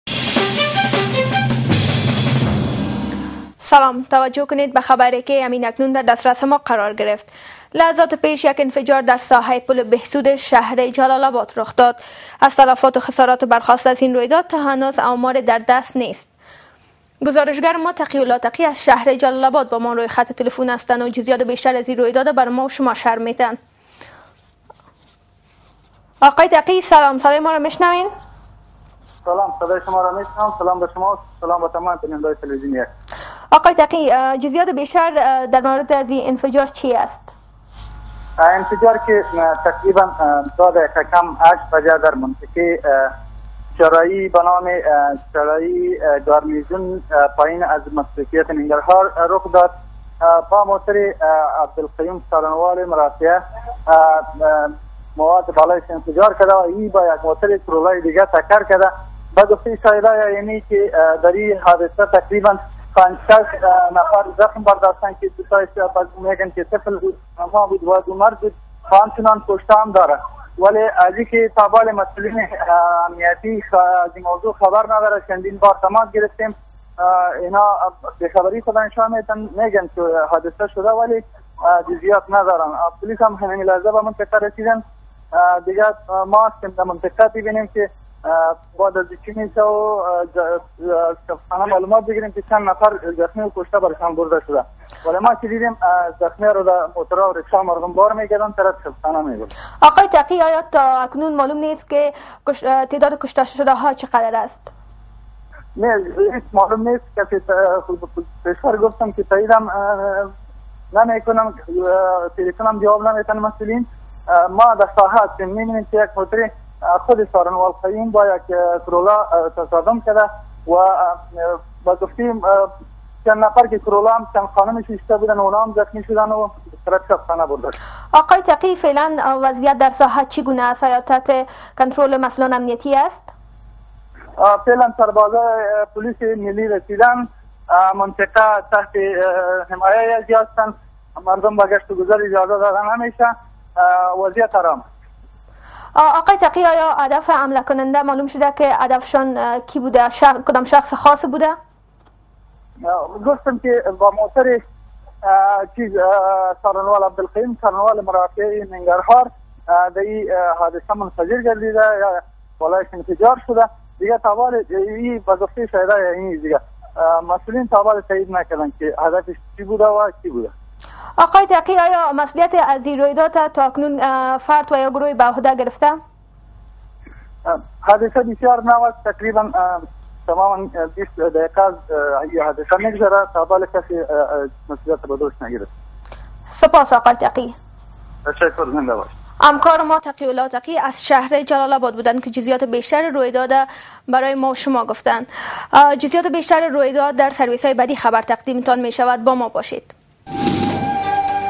1TV breaking News
جزییات رویداد انفجار در منطقه‌ی پل بهسود، از مربوطات شهر جلال آباد